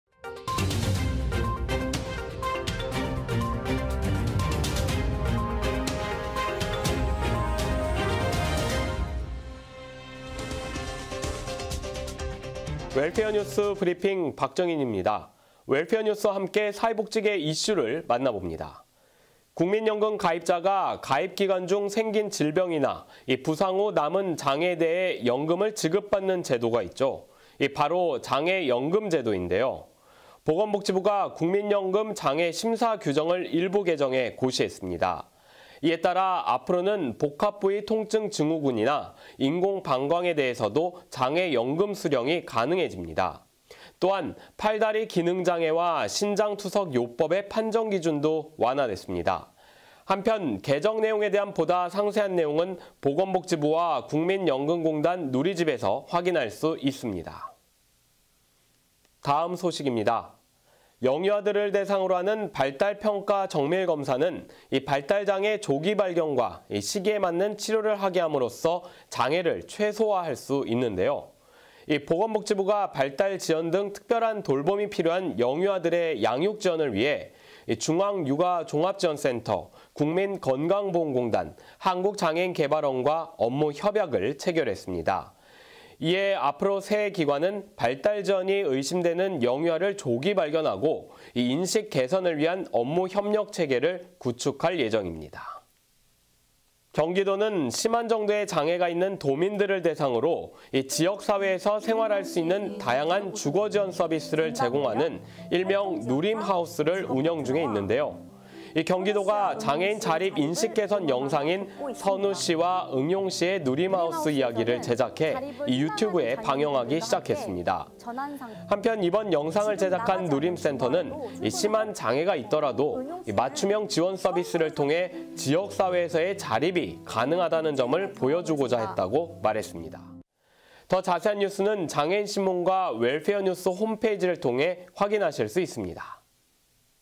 복지TV뉴스24 _ 웰페어뉴스 / 2023.01.04.
1. 보건복지부, ‘국민연금 장애심사규정’ 고시

2. 발달지연 영유아 ‘연속 지원체계’ 구축

3. 경기 누림센터, 장애인 자립 인식개선 영상 제작